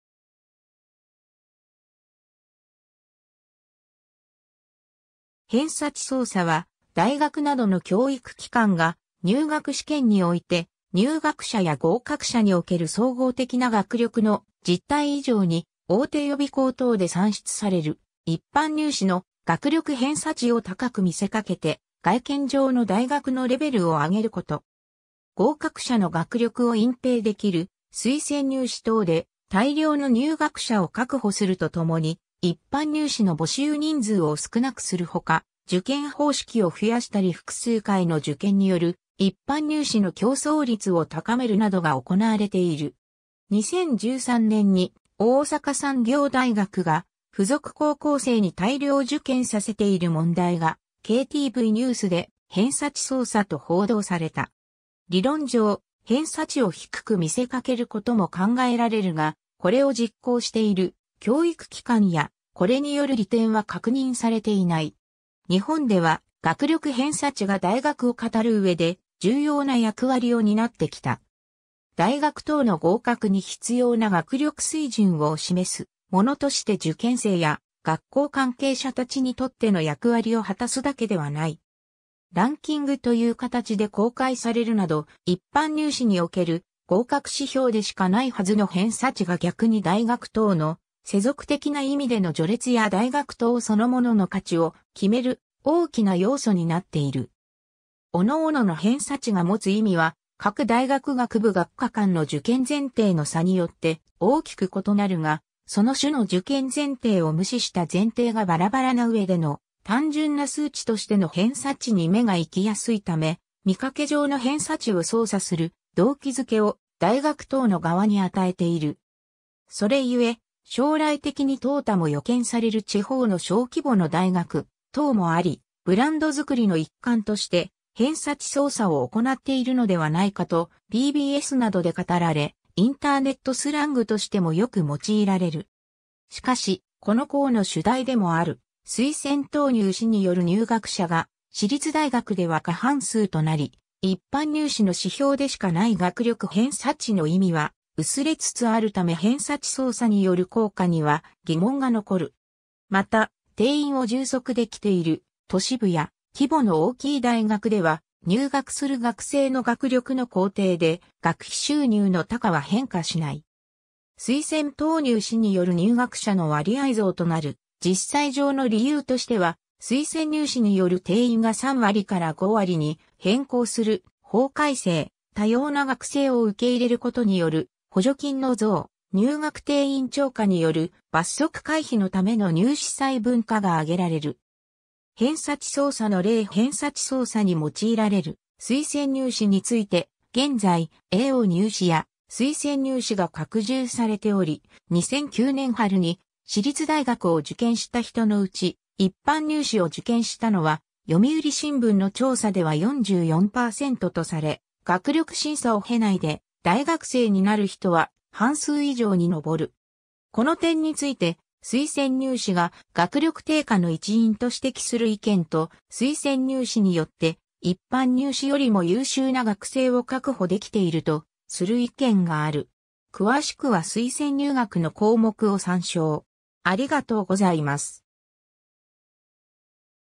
偏差値操作
偏差値操作（へんさちそうさ）は、大学などの教育機関が入学試験において、入学者や合格者における総合的な学力の実態以上に、大手予備校等で算出される一般入試の「学力偏差値」を高く見せかけて外見上の大学のレベルを上げること。
合格者の学力を隠蔽できる推薦入試等で大量の入学者を確保するとともに、一般入試の募集人数を少なくする他、受験方式を増やしたり複数回の受験による一般入試の競争率を高めるなどが行われている。
 2013年に大阪産業大学が附属高校生に大量受験させている問題がKTVニュースで偏差値操作と報道された。
理論上、偏差値を低く見せかけることも考えられるが、これを実行している教育機関やこれによる利点は確認されていない。
 日本では「学力偏差値」が大学を語る上で、重要な役割を担ってきた。
大学等の合格に必要な学力水準を示すものとして受験生や学校関係者たちにとっての役割を果たすだけではない。
ランキングという形で公開されるなど、一般入試における合格指標でしかないはずの偏差値が逆に大学等の世俗的な意味での序列や大学等そのものの価値を決める大きな要素になっている。
各々の偏差値が持つ意味は各大学学部学科間の受験前提の差によって大きく異なるが、その種の受験前提を無視した前提がバラバラな上での単純な数値としての偏差値に目が行きやすいため、見かけ上の偏差値を操作する動機づけを大学等の側に与えている。
それゆえ、将来的に淘汰も予見される地方の小規模の大学等もあり、ブランド作りの一環として偏差値操作を行っているのではないかと、BBSなどで語られ、インターネットスラングとしてもよく用いられる。
 しかし、この項の主題でもある、推薦等入試による入学者が私立大学では過半数となり、一般入試の指標でしかない学力偏差値の意味（対象となる人数）は薄れつつあるため偏差値操作による効果には疑問が残る。
また、定員を充足できている都市部や規模の大きい大学では、入学する学生の学力の高低で学費収入の多寡は変化しない。
推薦等入試による入学者の割合増となる実際上の理由としては、推薦入試による定員が3割から5割に変更する法改正、多様な学生（編入生、社会人、障害者、留学生等）を受け入れることによる補助金の増、入学定員超過による罰則回避のための入試細分化（回数を細かくすればするほどリスクが下がる）が挙げられる。
 偏差値操作の例 偏差値操作に用いられる推薦入試について、現在、AO入試や推薦入試が拡充されており、2009年春に私立大学を受験した人のうち、一般入試を受験したのは『読売新聞』の調査では44%とされ、学力審査を経ないで大学生になる人は半数以上に上る。
この点について、推薦入試が学力低下の一因と指摘する意見と、推薦入試によって一般入試よりも優秀な学生を確保できているとする意見がある。
詳しくは推薦入学の項目を参照。